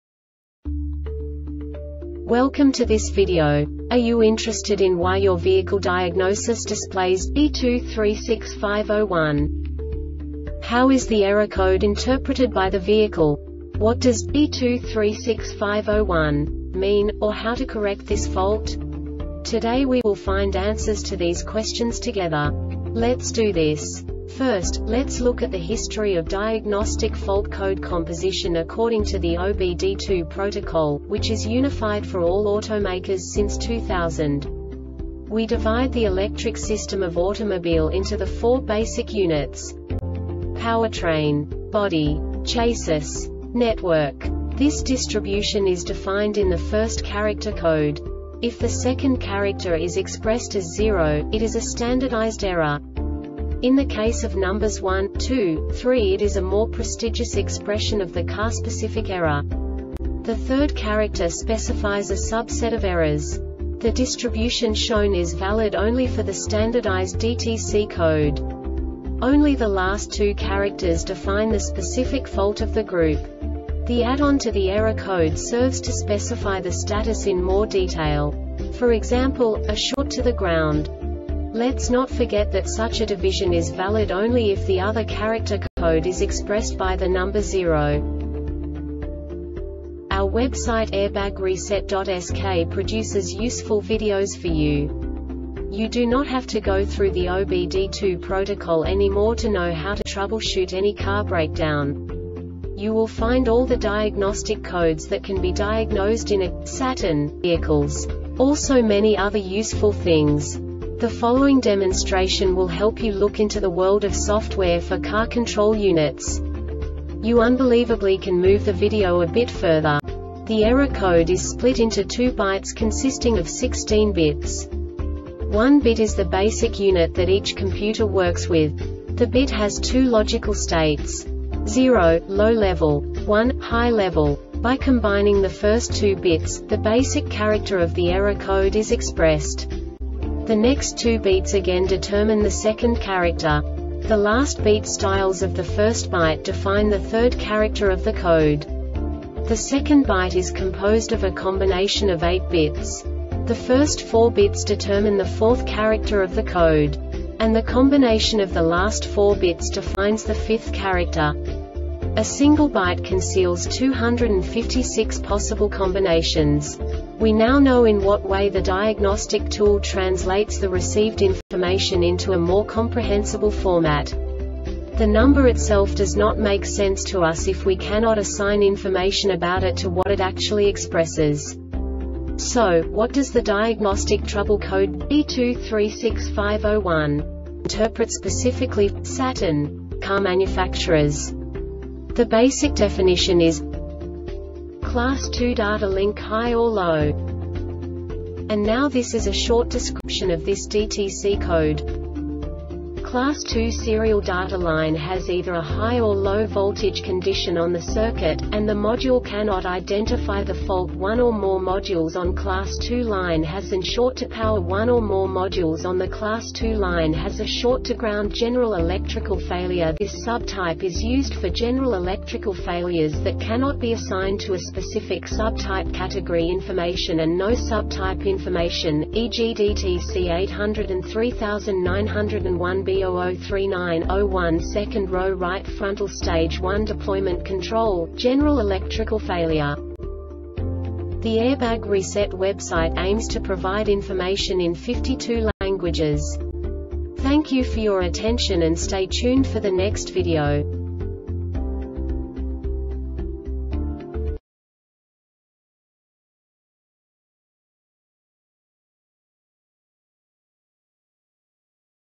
DTC Saturn B2365-01 Short Explanation
Contents:
0:21 Basic DTC analysis according to OBD2 protocol standard.
1:48 Insight into programming
2:58 A diagnostically understandable description of the Diagnostic Trouble Code
3:05 Basic error definition

Make:
Saturn
Code:
B2365 01
Definition:
Class 2 Data Link High or Low
Cause: